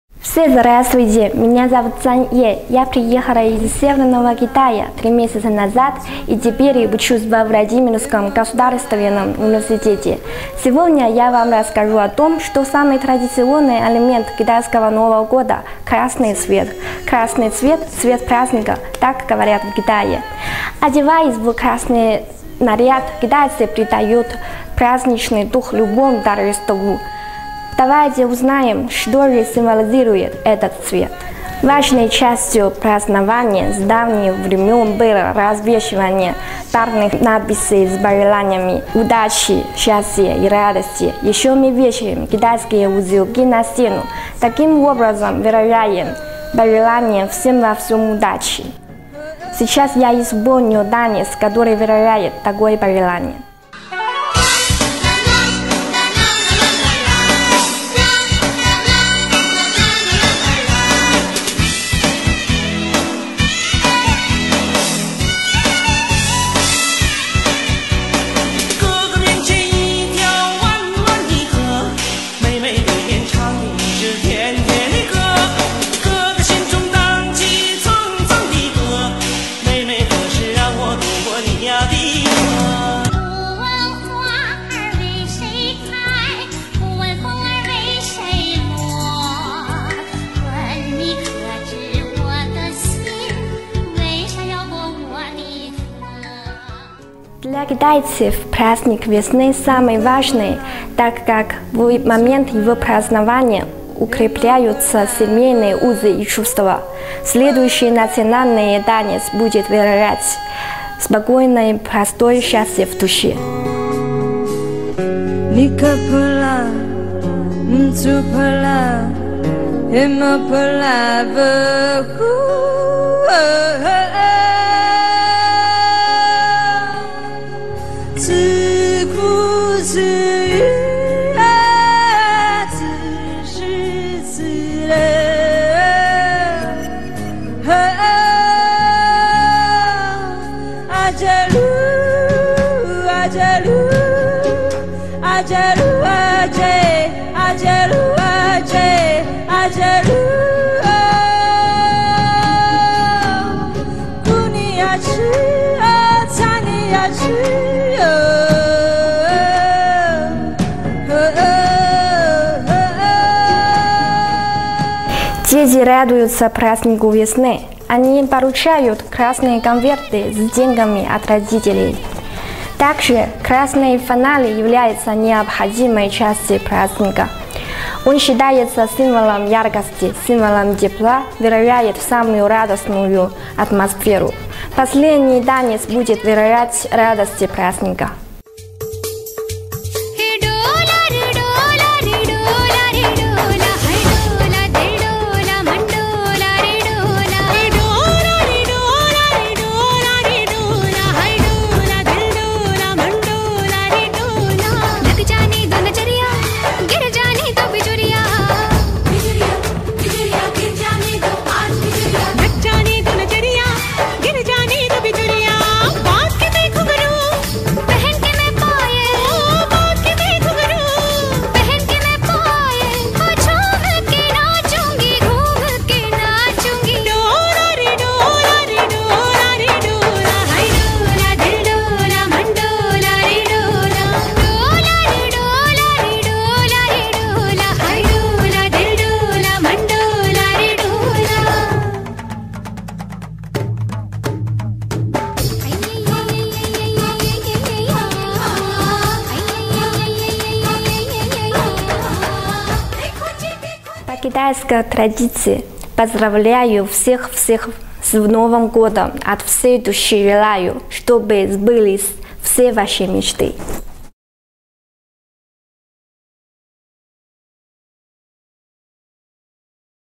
Цань Е, КНР
Второй онлайн-фестиваль дружды "Вместе встретим Новый год!", 19 января 2013 г., Московский государственный университет имени М.В.Ломоносова